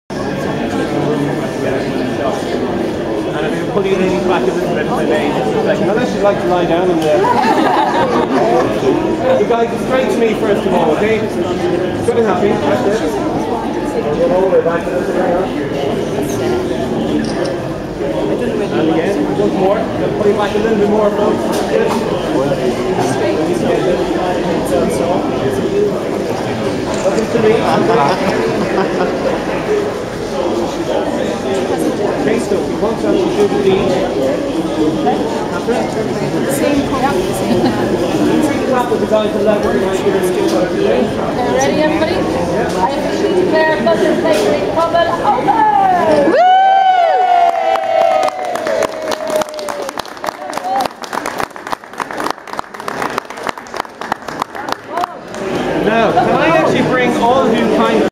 Kathryn Thomas officially opens Thunders Bakery Crumlin
Rte presenter cut the ribbon to officially open the newest Thunders bakery store in Ashleaf Shopping Centre, Crumlin, Dublin 12.